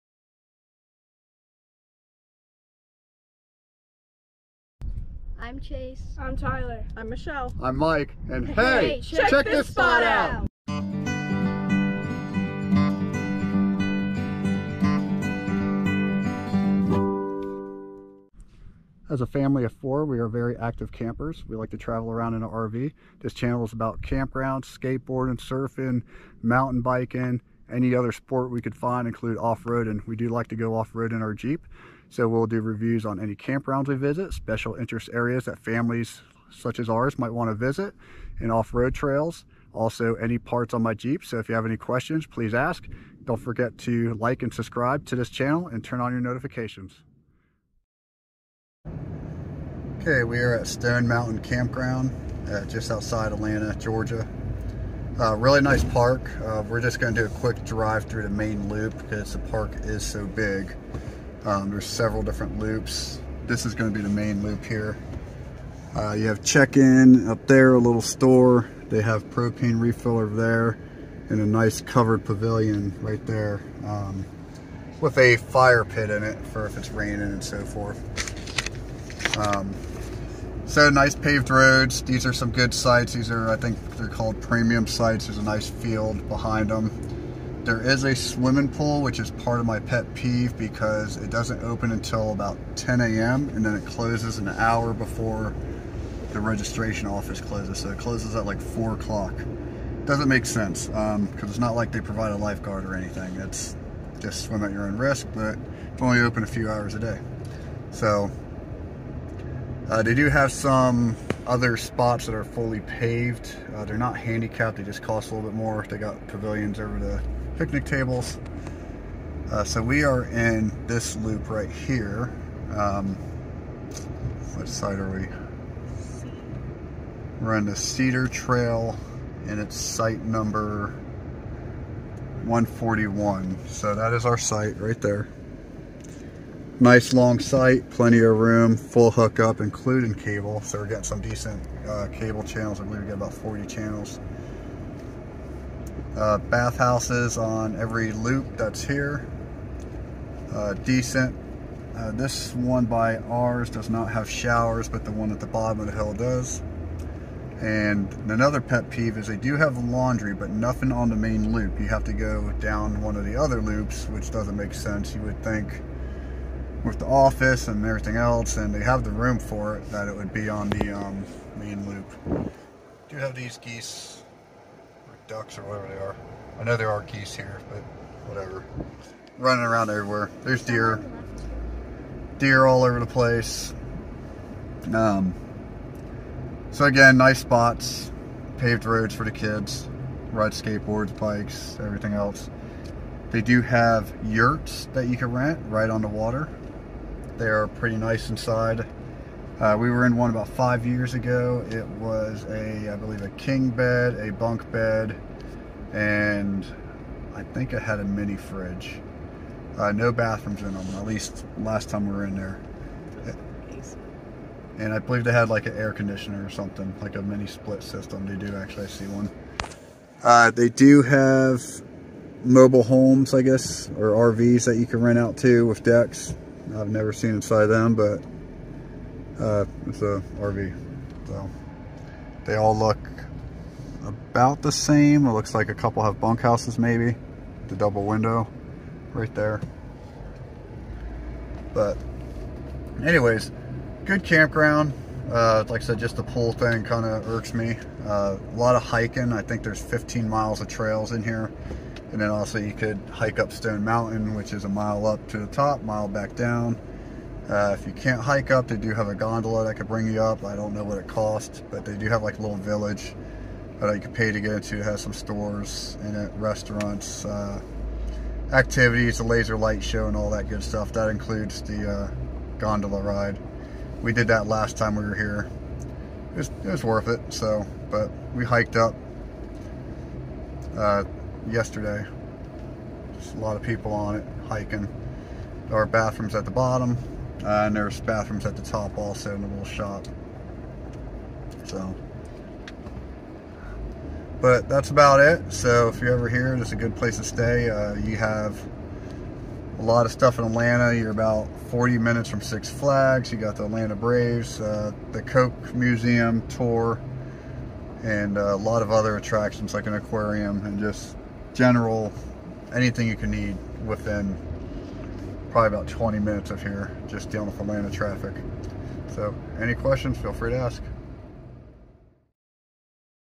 Stone Mountain Park Campground Atlanta GA
Quick review on our loop at Stone Mountain.